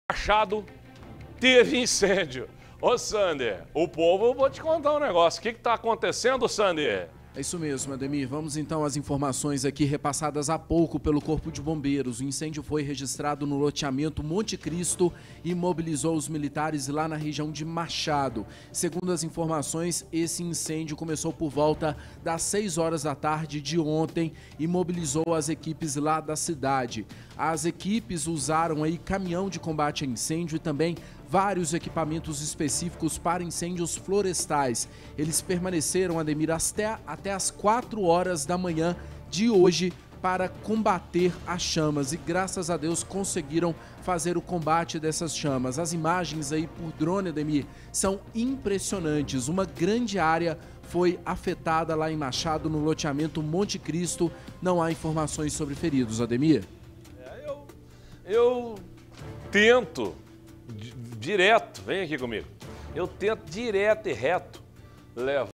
Fogo se alastra em vegetação em Machado
FOGO SE ALASTRA EM VEGETAÇÃO EM MACHADO | Veja as imagens de um fogo em uma vegetação em Machado.